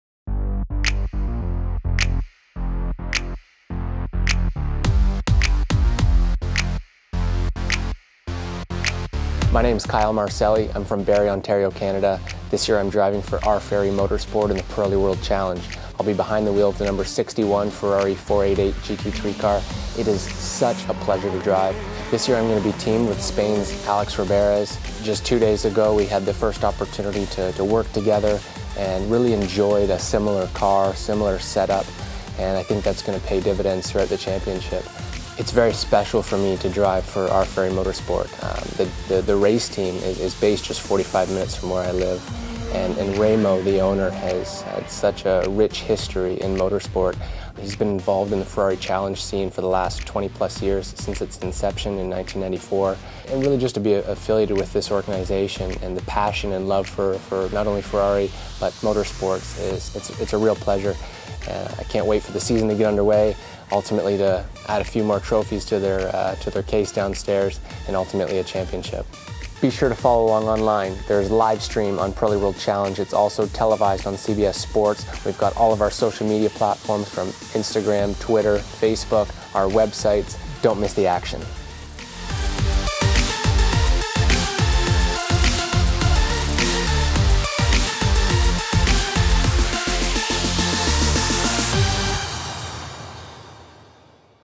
Driving the Ferrari 488 GT3 Race Car
Very happy to join R.Ferri Motorsport/R.Ferri Racing and Alex Riberas at the wheel of the #61 Ferrari 488 GT3 for the 2017 Pirelli World Challenge SprintX Championship! Let's go!